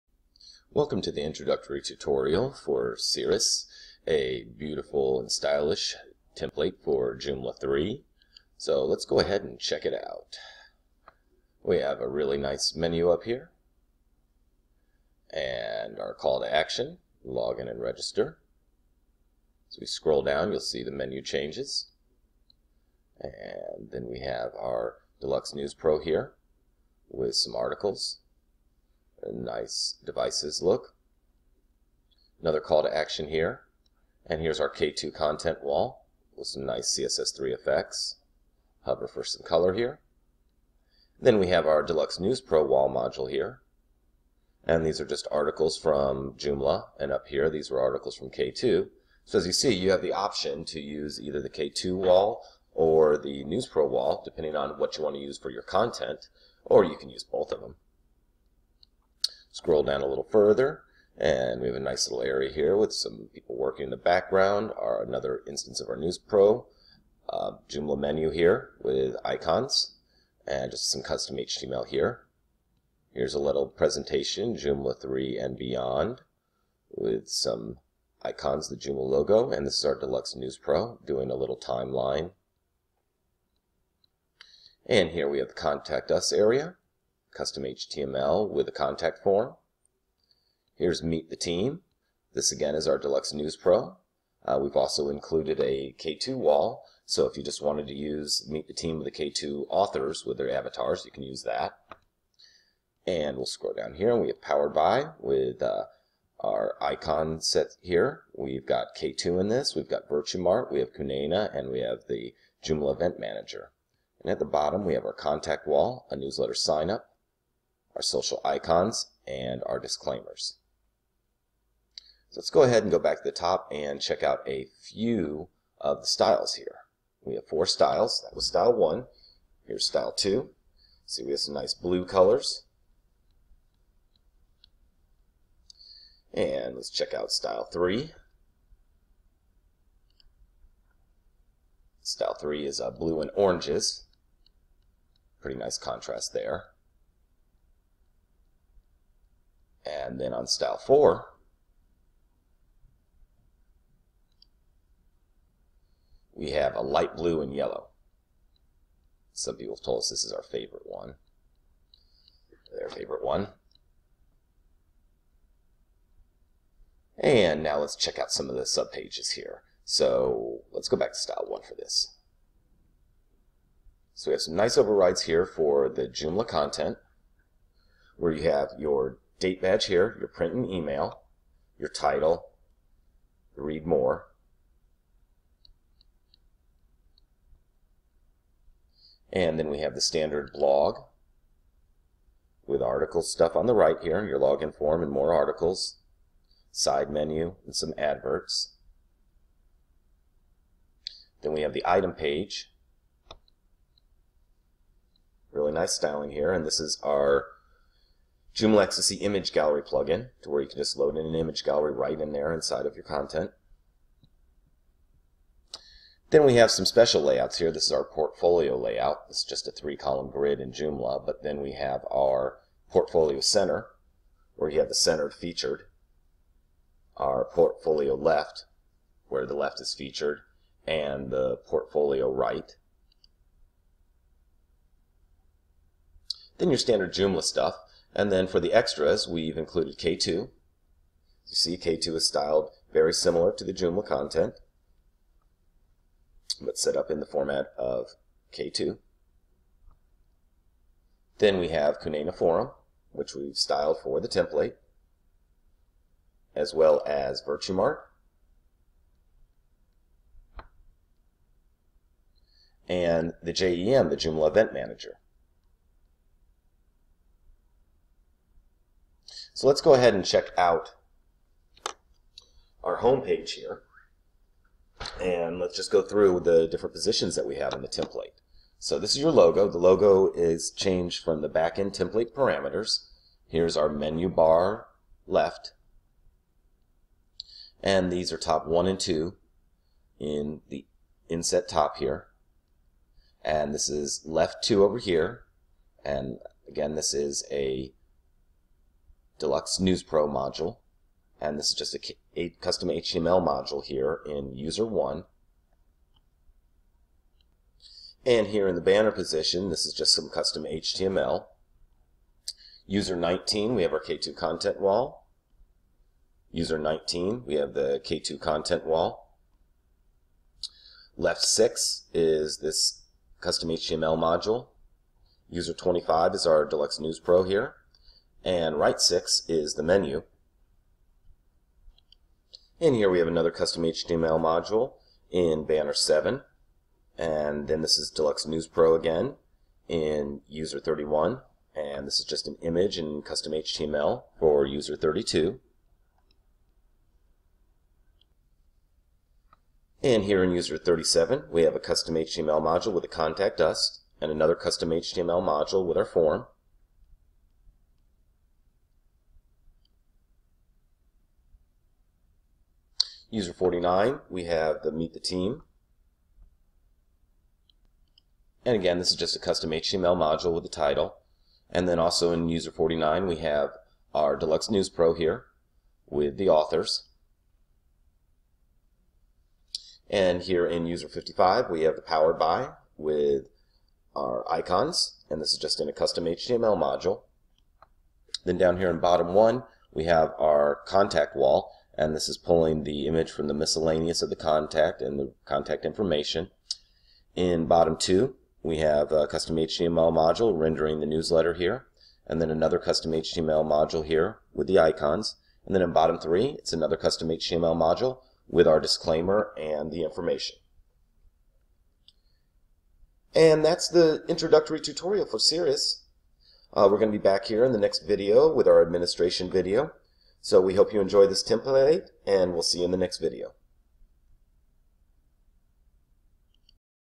Sirus - A Pro Joomla Template - Intro Tutorial
Sirus is a beautiful and flexible template for Joomla 3 using the latest trends in web design. Clean and content driven, Sirus includes built-in support for K2, Kunena, Virtuemart, and JEM, plus some of your favorite content walls from our collection with ready-to-go templates. Completing the package are Image Gallery plugin and Modal plugins to enhance your content with additional features. No matter the size of your website, Sirus will take it to the top!